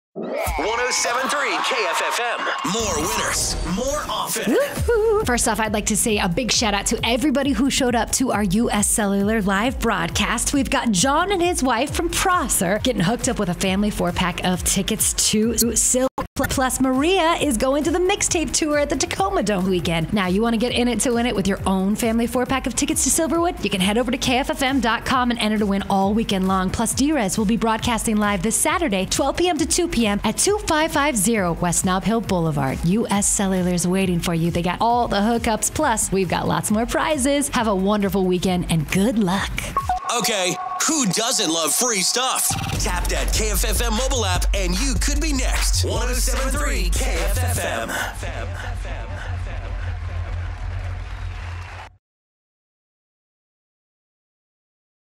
US Cellular Remote Winners
What an awesome time we had out at US Cellular and we do it again tomorrow! Details found at kffm.com